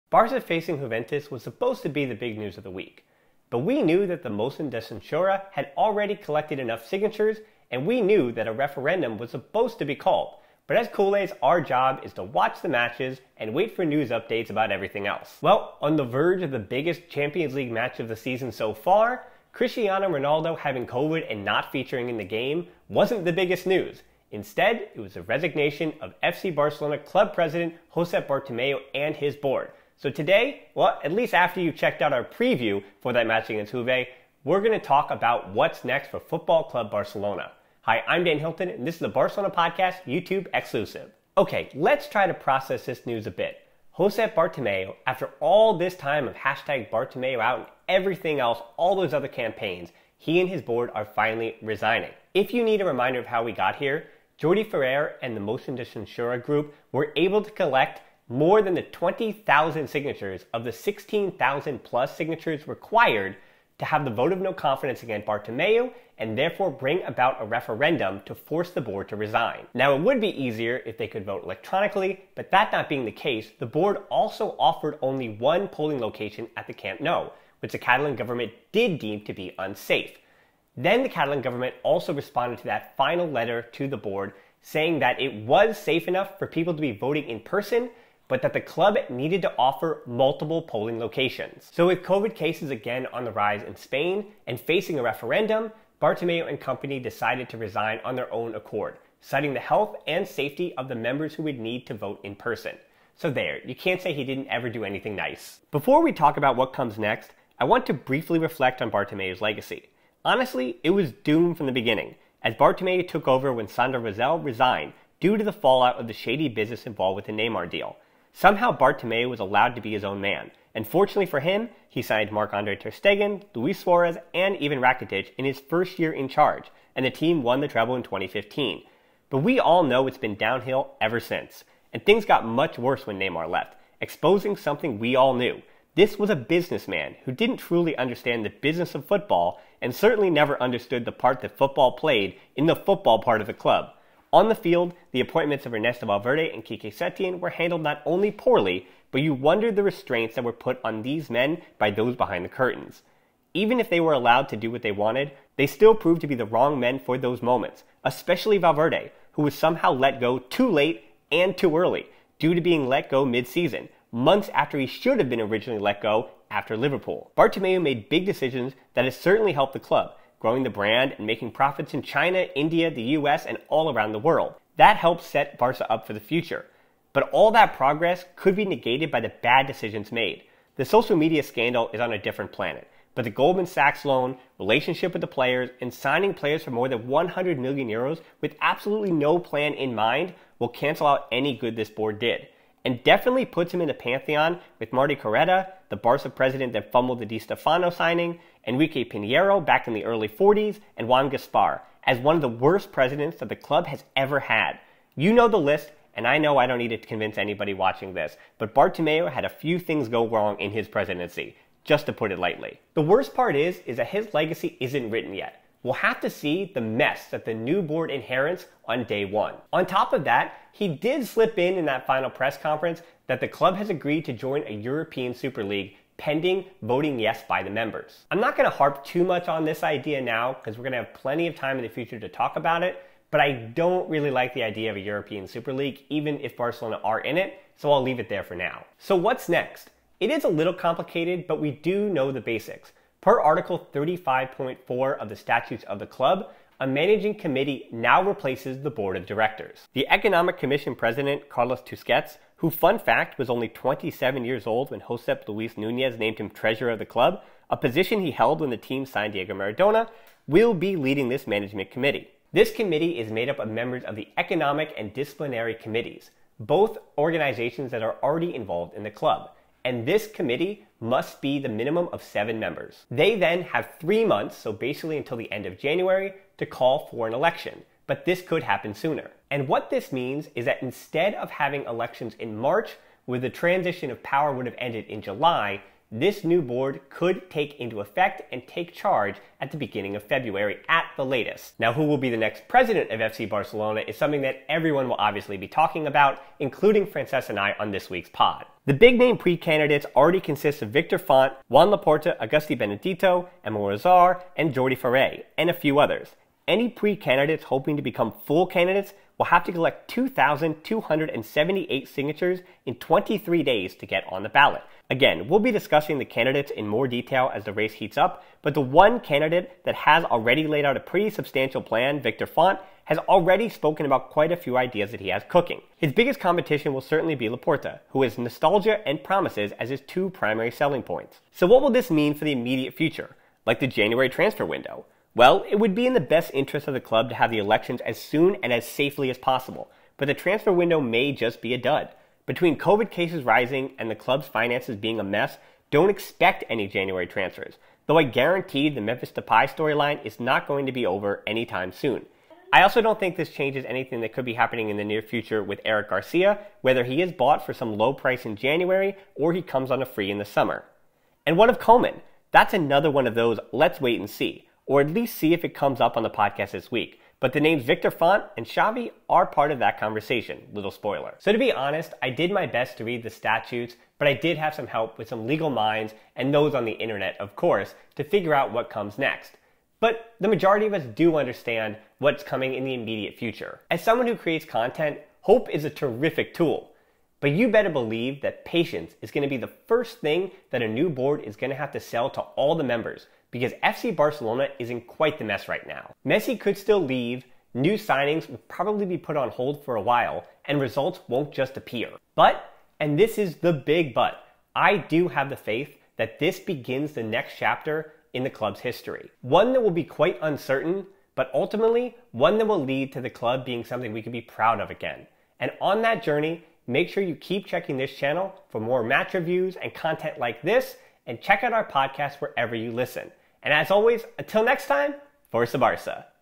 Bartomeu Resigns as FC Barcelona President | Electing a New Board | Explainer
Josep Bartomeu has resigned as FC Barcelona President along with the rest of the board. A few questions to consider:

- What is Bartomeu's legacy?
- Who takes over before a new board is elected?
- When will the new board be elected?
- Who are the favorites to be the next president?